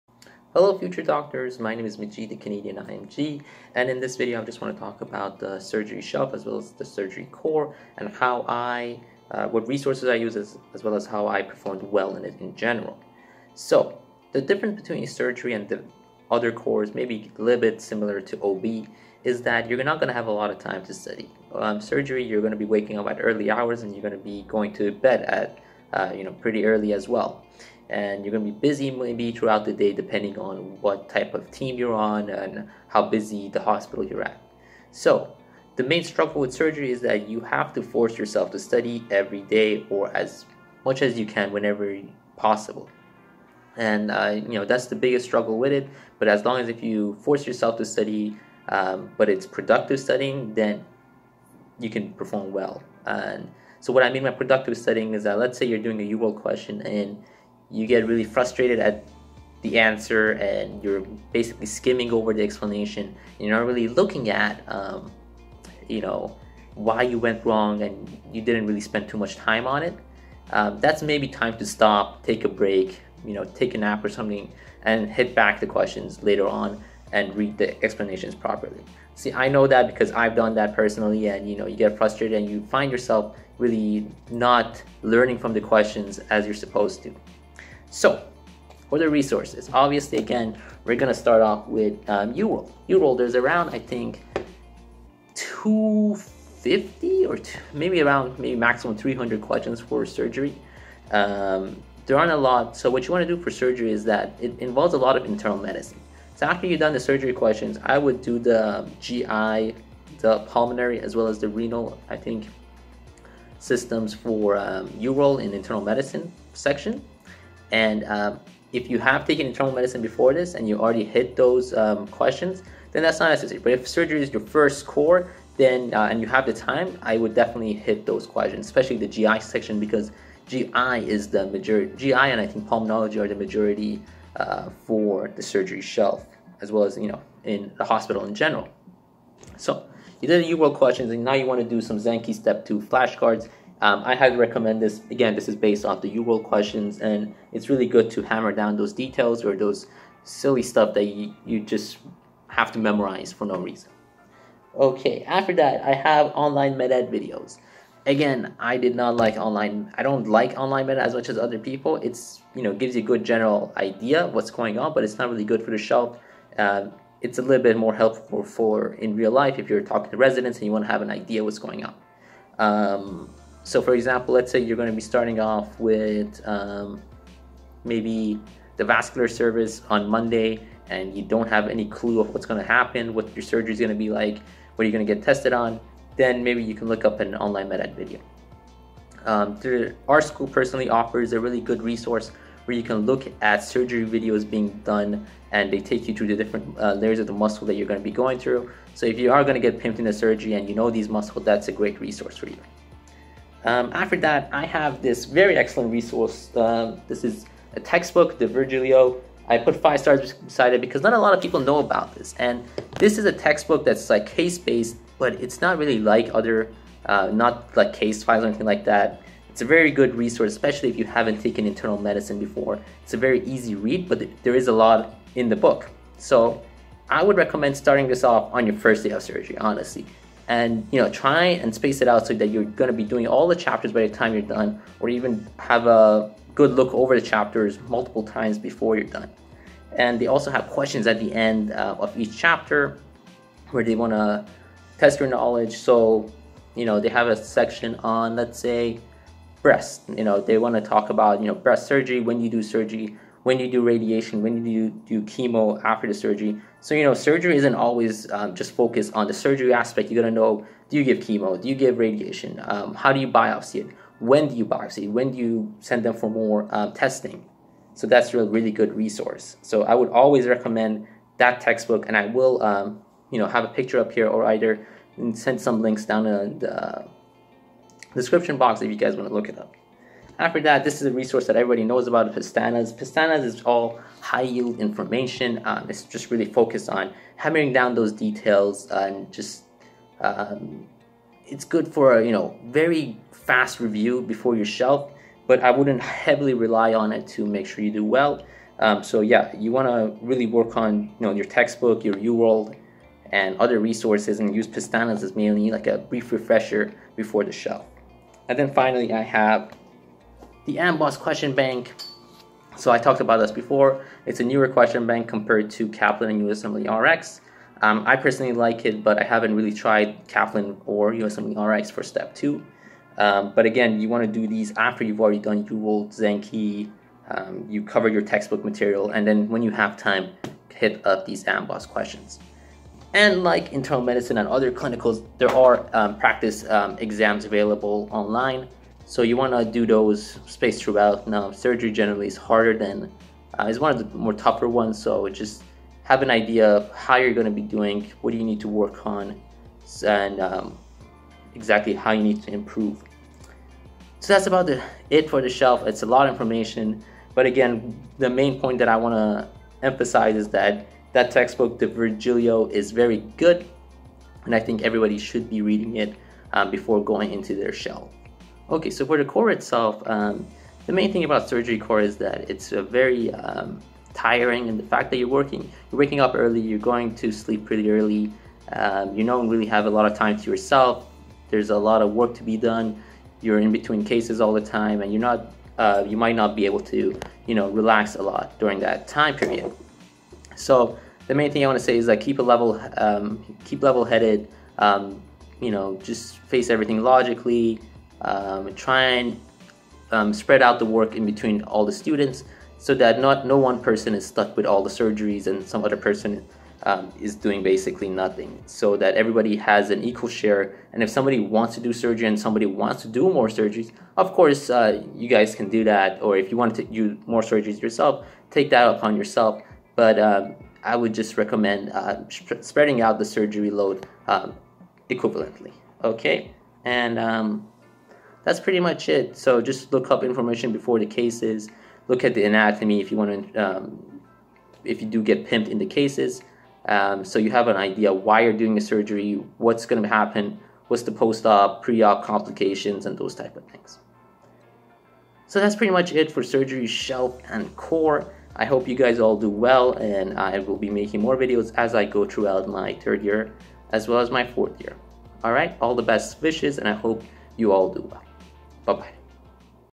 Surgery Clerkship and Shelf Exam - Study Tips and Resources | RUSM
Hi everyone,

The main struggle with surgery is the hours. Just make sure you are able to study something everyday and stay focused.

Resources mentioned:

Zanki

OME

De Virgilio

Pestanas

AMBOSS

All the best wishes,

Majid